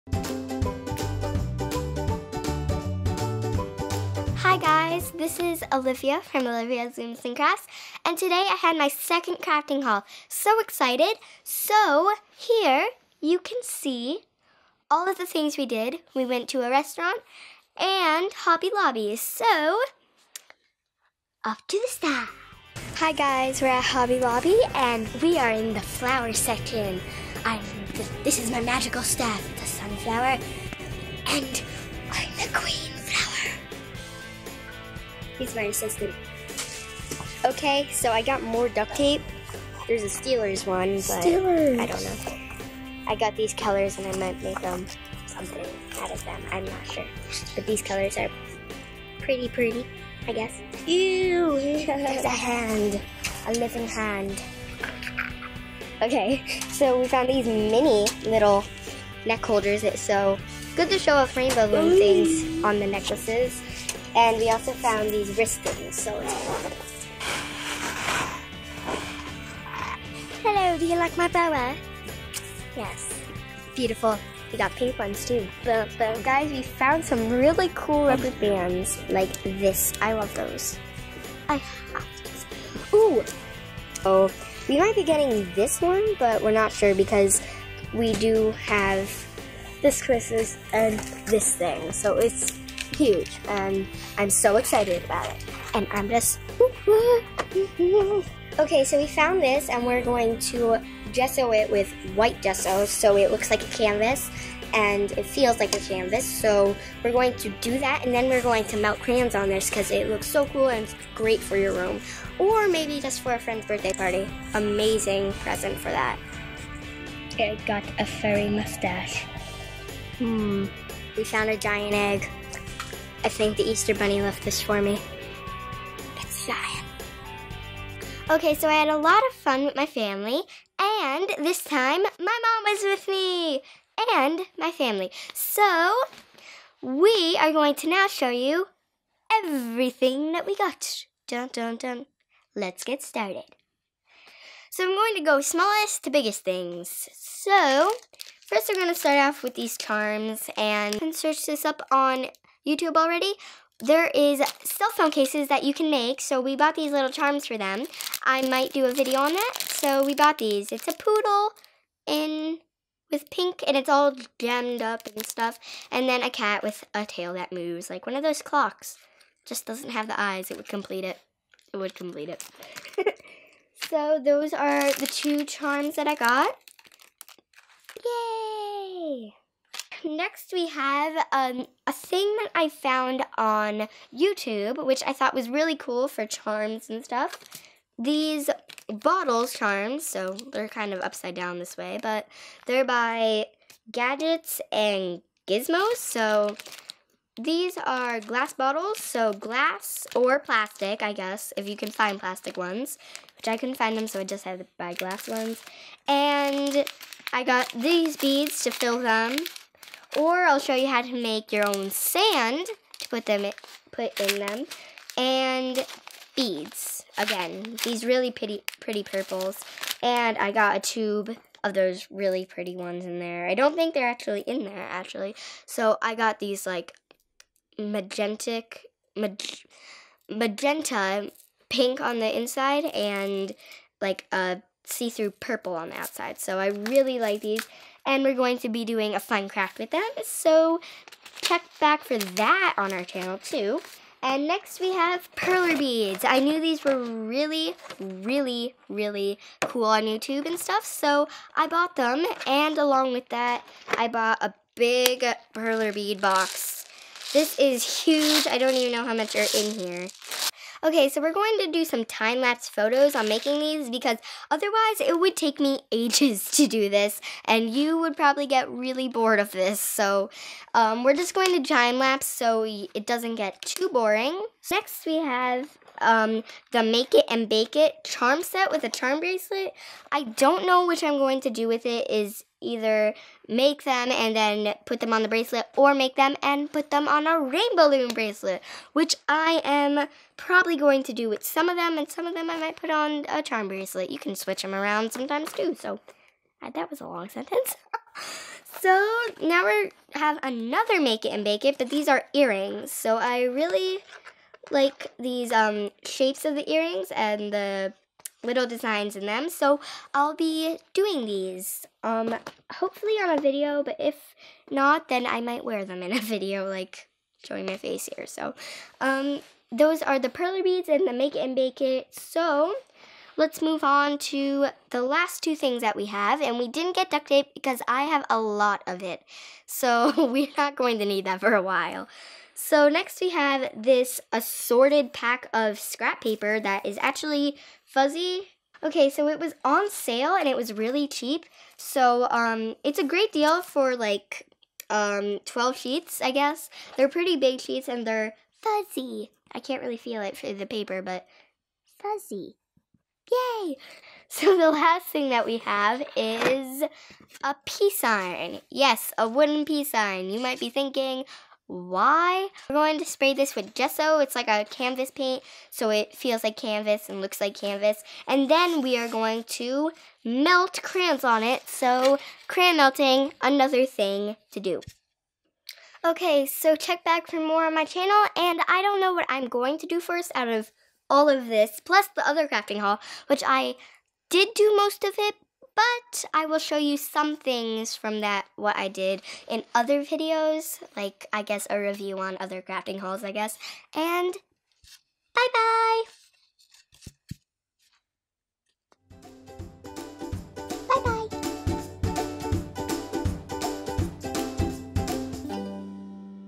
Crafting Haul - Hobby Lobby
Hi everyone, This is Olivia. I just did my second crafting haul. So much fun! I hope you like this video. Peace, Olivia